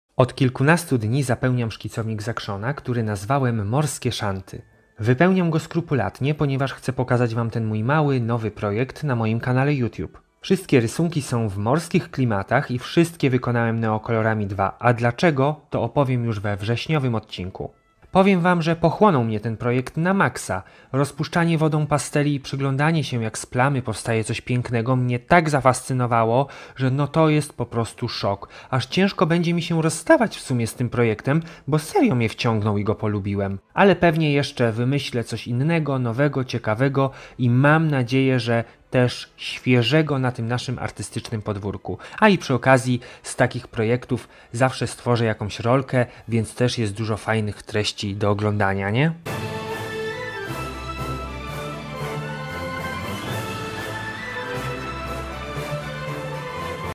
NAJGORSZY SZKICOWNIK Z ACTION ! WYPEŁNIAM GO RYSUNKAMI ?!
Najgorszy szkicownik z Action został przeze mnie wypełniony. Będzie nosił nazwę "morskie szanty". Właśnie w takich klimatach będą wszystkie ilustracje! ⛵💙